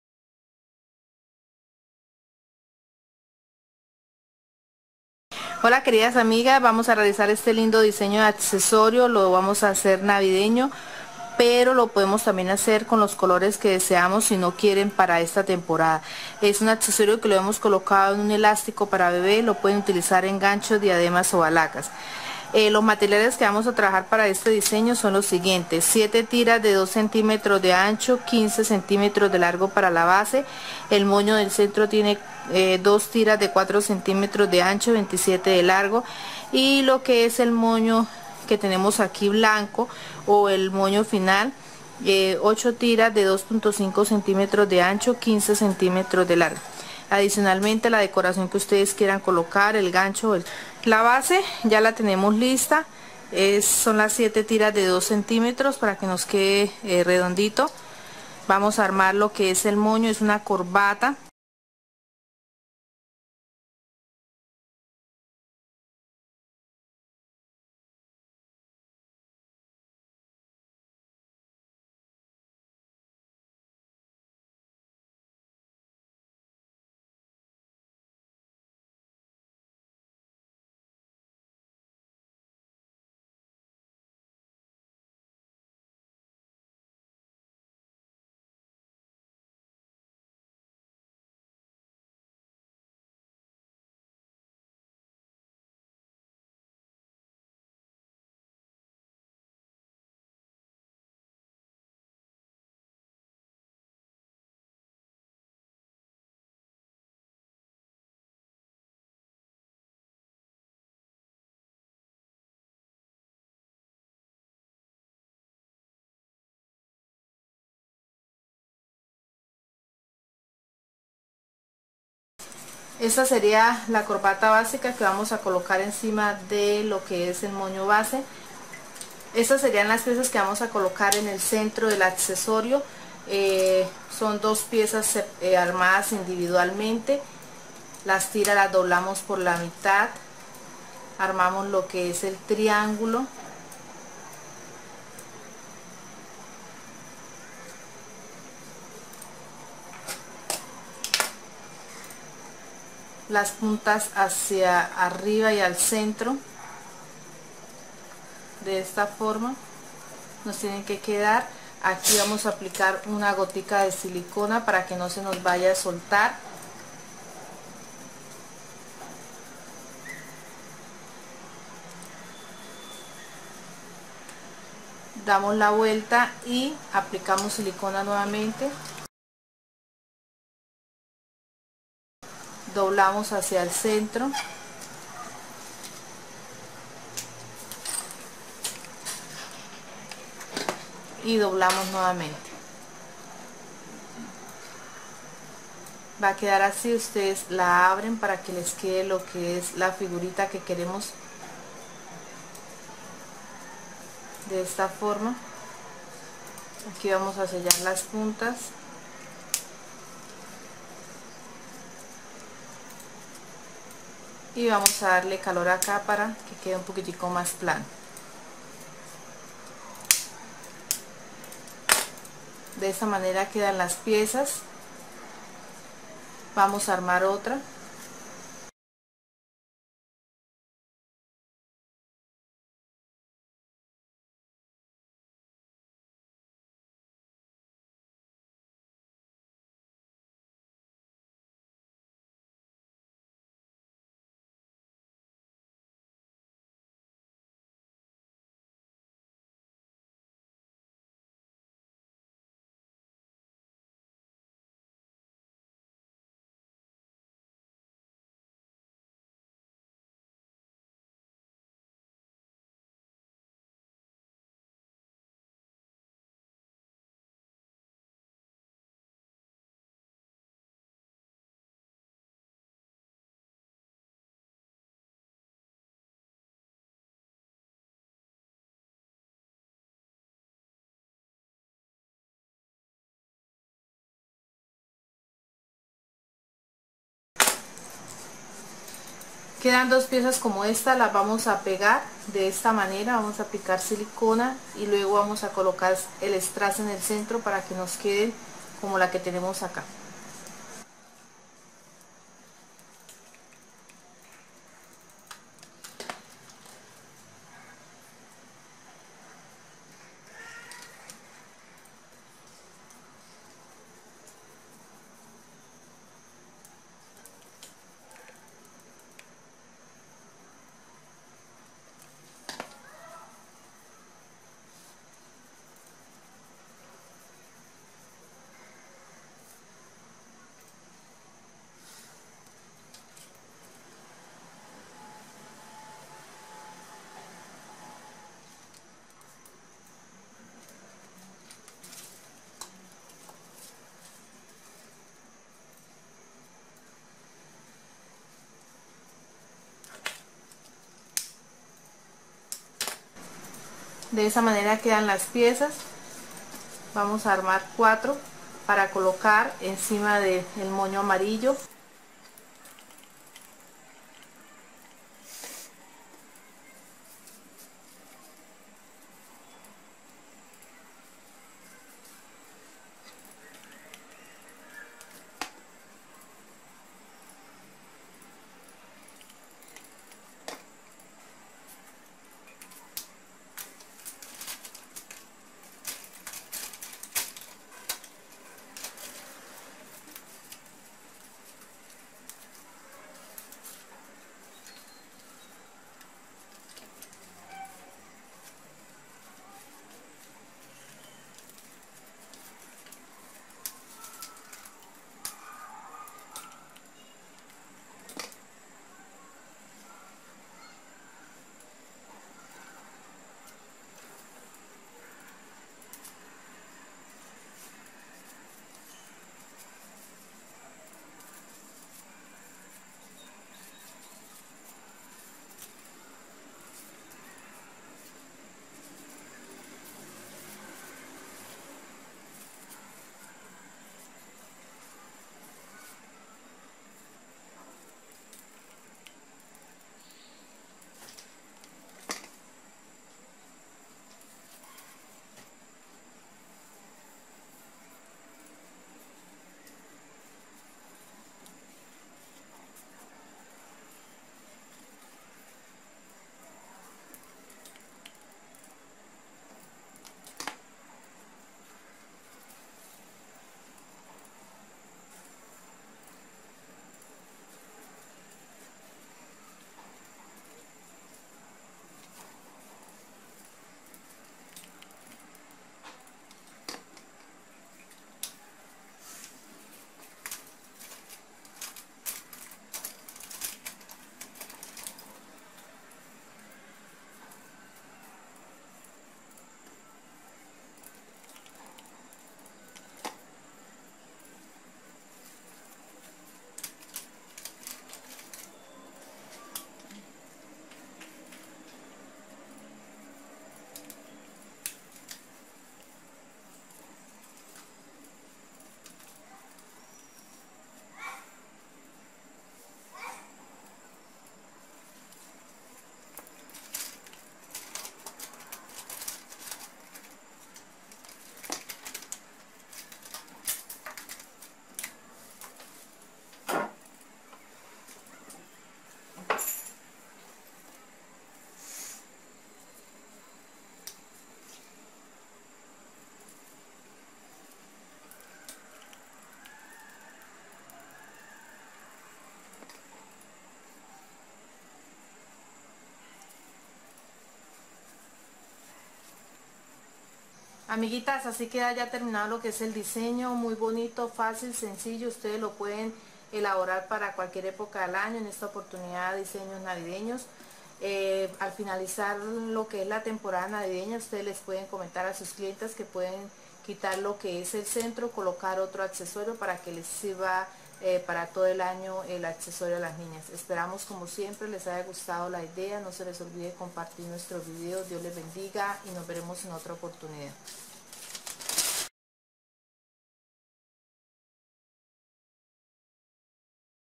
Tiaras colores de Navidad para Bebe, Como Hacer Moños
Tutorial de moños con material  economico  y facil de conseguir
ideas para toda ocasion  o época del año
para pegar en ganchos diademas o balacas